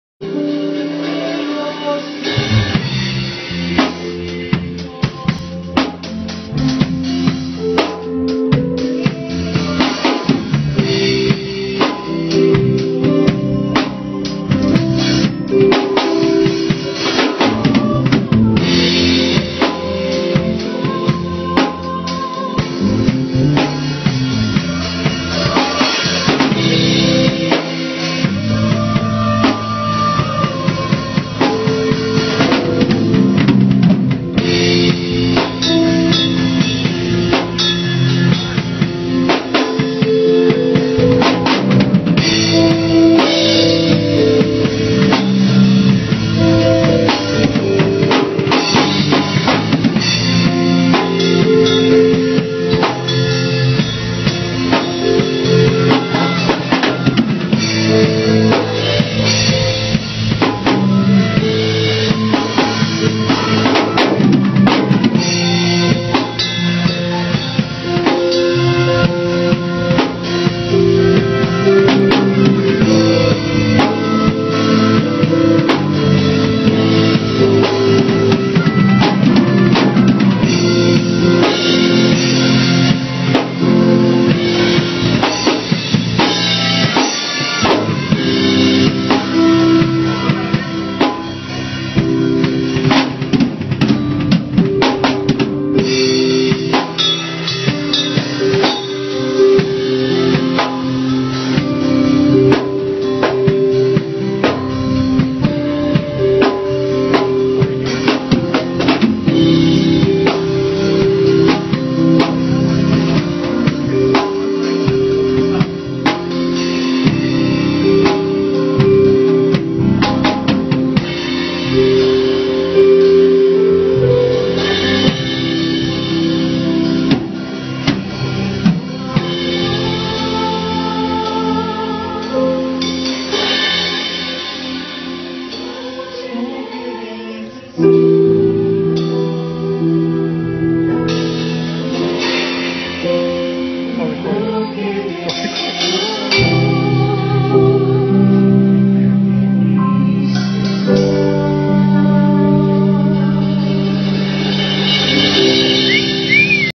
Music rehearsal at church 2nd vid.
Me on the drums just trying to get the song down. Sorry about the image quality but the sound is nice.